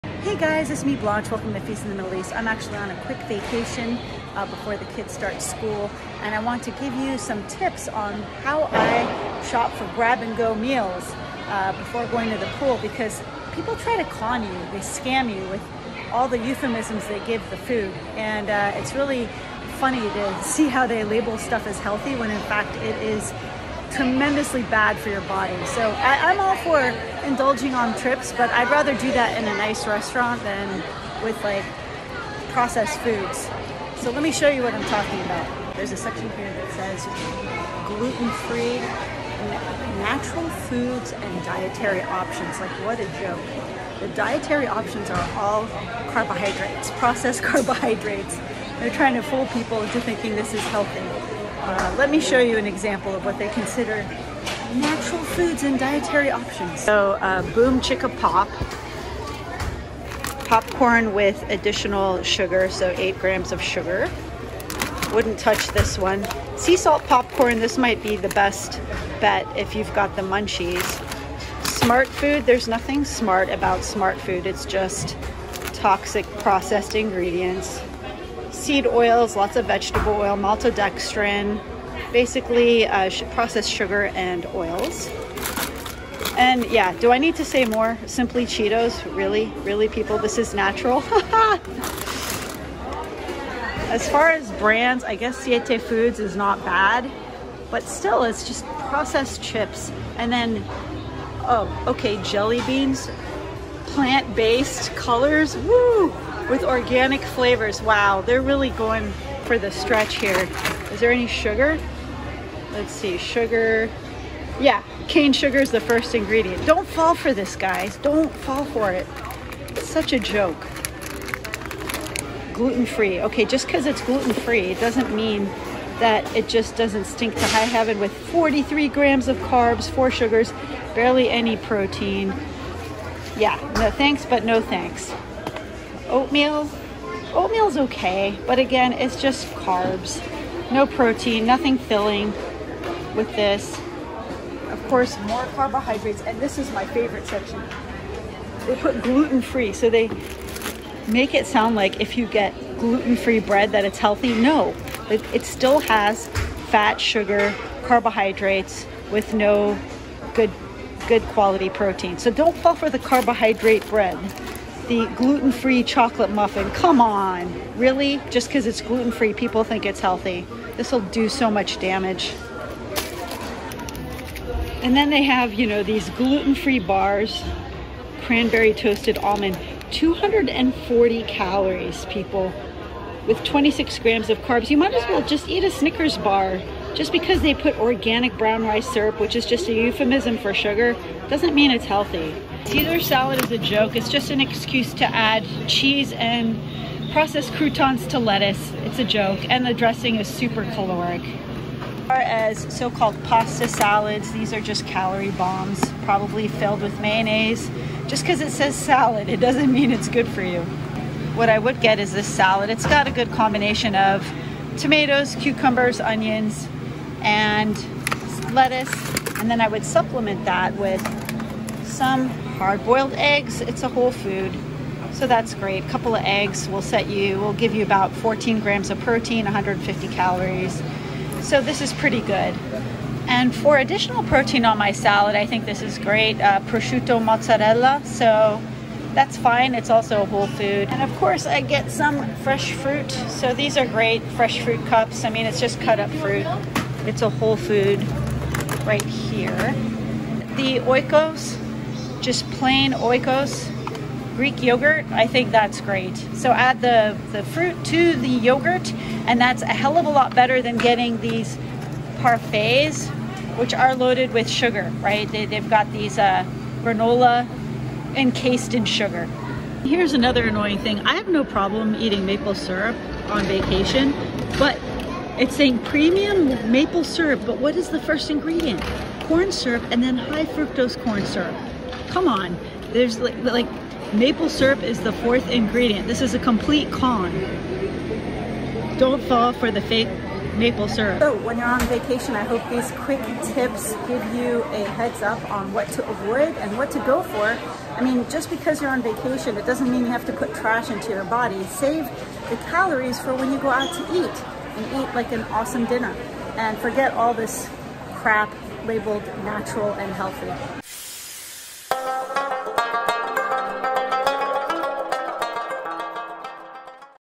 HOW THEY LIE: "HEALTH" FOOD SCAMS and CONS ON the GO
Whether you are on a road trip, taking a vacation, or out on business, there are processed food traps everywhere, disguised as health food! In this video I show some quick tips on how to spot them, and how to make the best choices to feel your best, prevent fat gain, and get maximum nutrition on the go!

GET $40 OFF YOUR OWN GUT HEALTH TEST KIT: TRYOMBRE.COM/BLANCHETV

AMAZON STORE